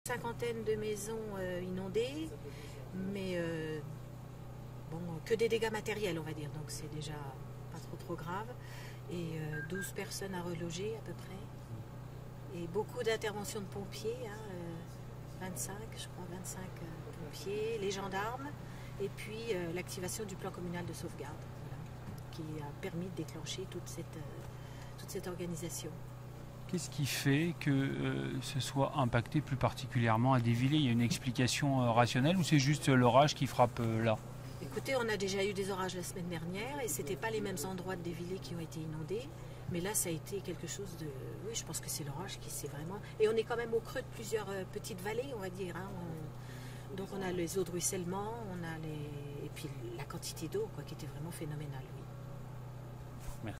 Des fortes précipitations ont inondé les communes de Deyvillers
Les Vosges ont connu un autre épisode orageux lundi 4 juin. Les fortes précipitations qui ont inondé les communes de Deyvillers et Jeuxey ont provoqué des dégâts dans les habitations.